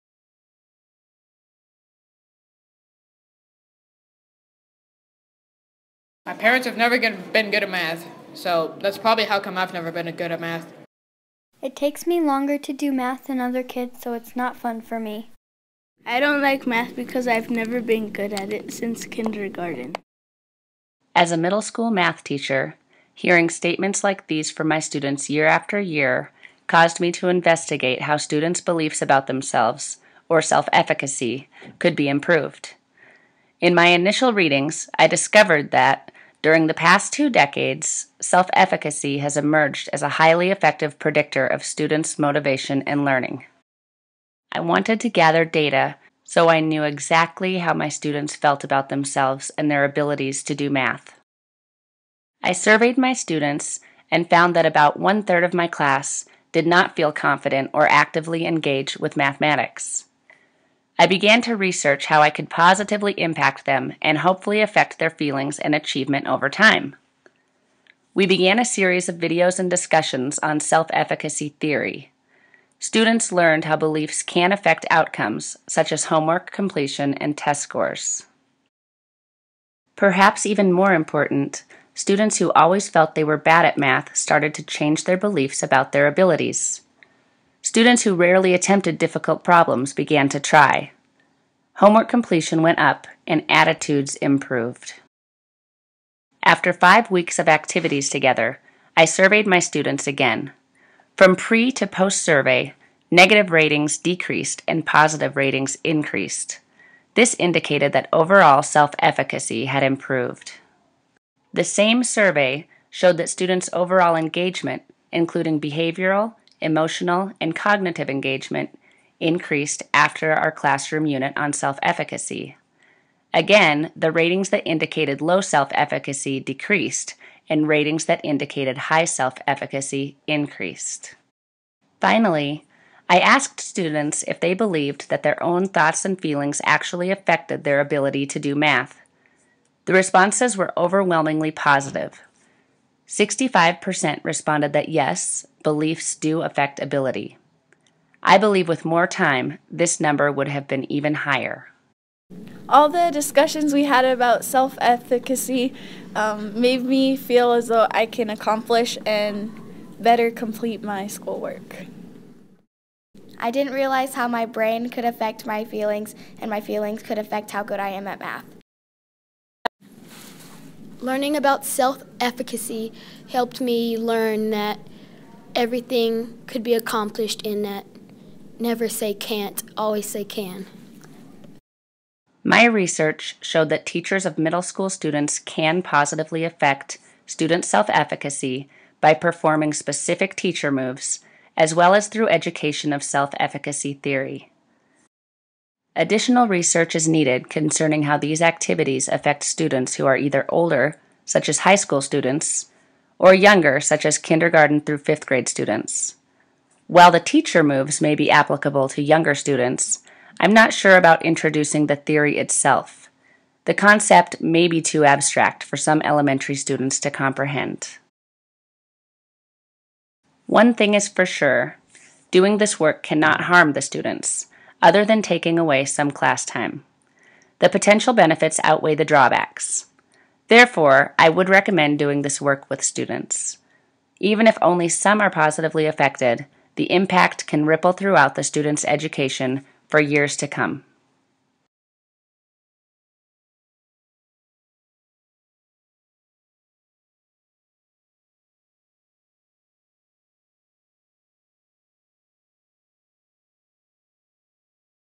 Self-Efficacy Action Research Project
I created this video to share some basic information about my action research report, completed for CU-Denver's Information and Learning Technologies graduate program.  I chose to research how self-efficacy could be improved for my middle school math students.  To view the full report, copy and paste this link: